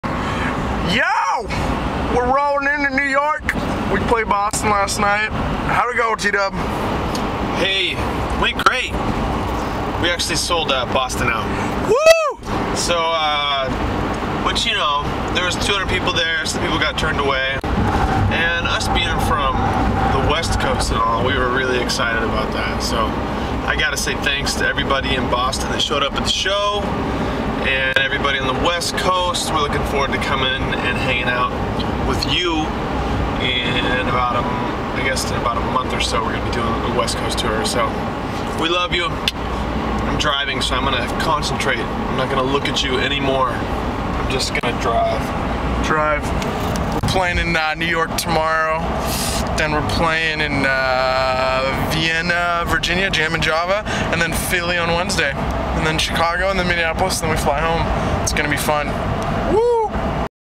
Boston to NYC
Tyrone Wells and Mark Chipello kick of their West Coast acoustic tour